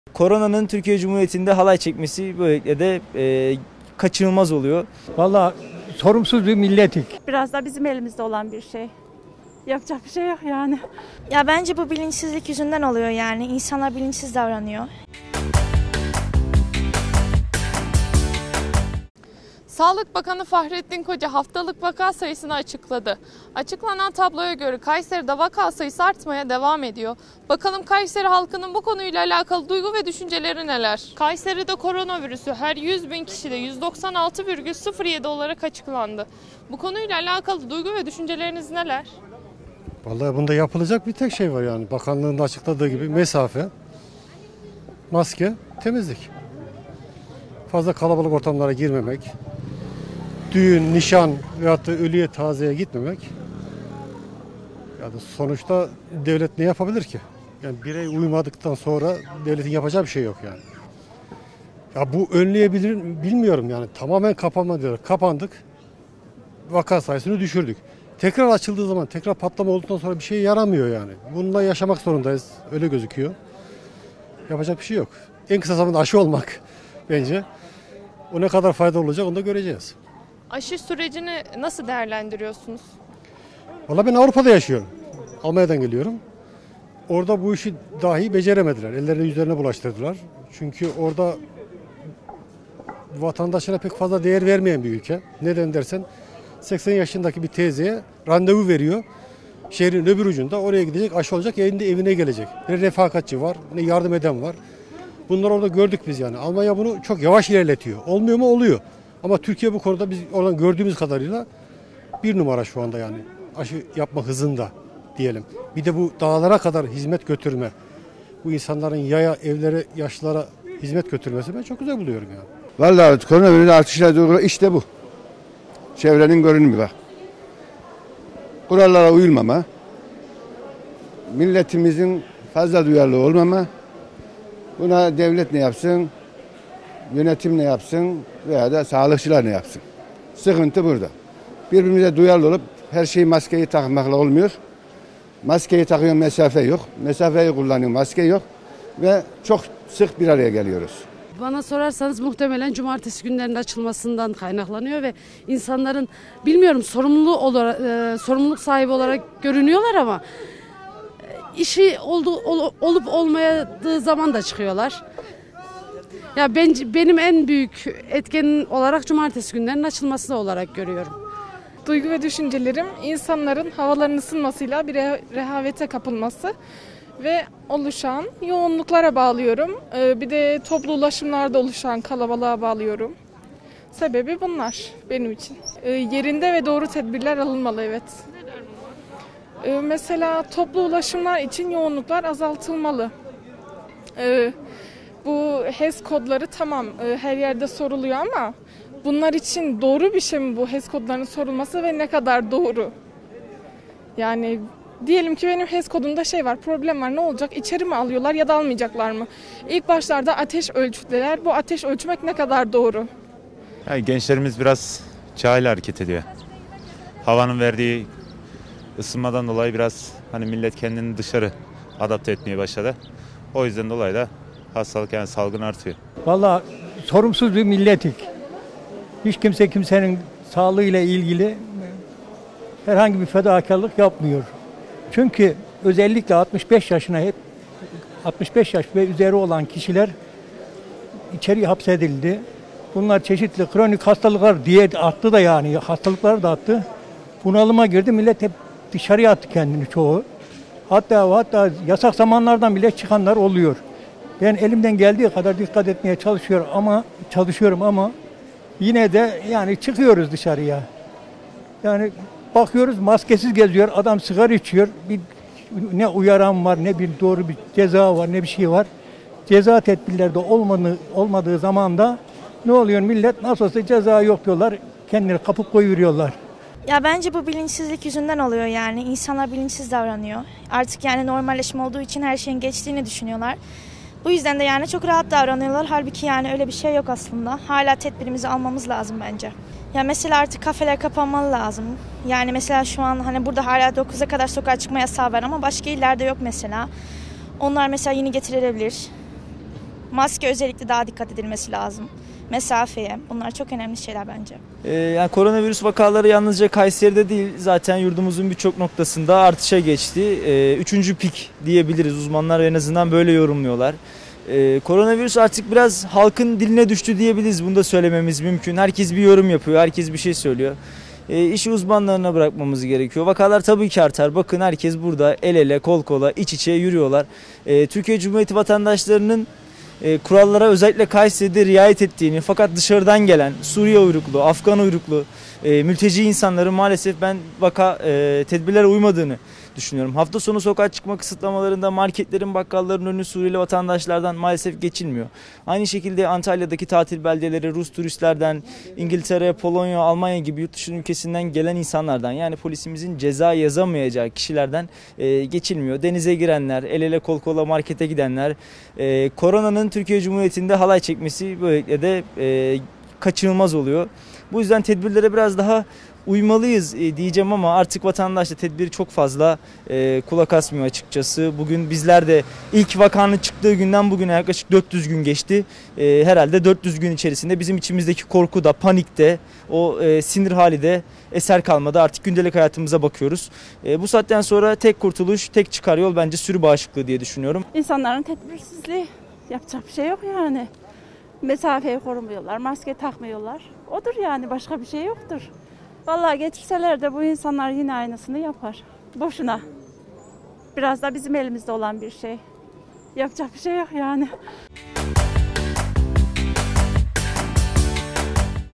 Kayseri'de Korkutan Vaka Artışı
Fahrettin Koca haftalık vaka tablosunu açıkladı. Açıklanan tabloya göre Kayseri'de vaka artışı devam ediyor. Son verilere göre Kayseri'de koronavirüsü vaka oranı her 100 bin kişide 196.07 olarak açıklandı. Artan vaka nedeniyle Kayseri'de uyarılar yapıldı.
Kayserimnet LafSokakta ekibi sokağa artan vaka artışlarını  sordu...